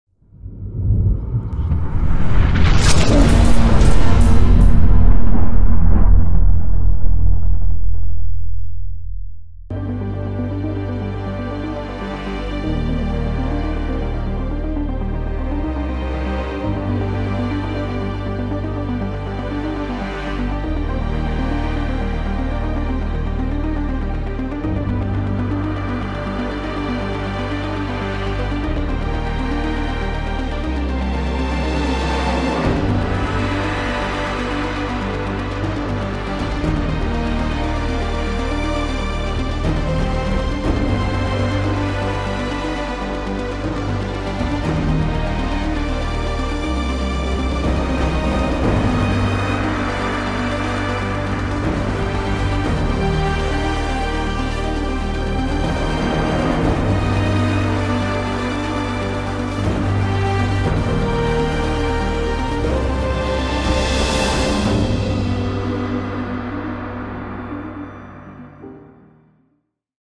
TORNADO TIMELAPSES NW Texas 4-23-2021
Time lapses of the tornadoes we intercepted on Friday 23 April 2021 in NW Texas!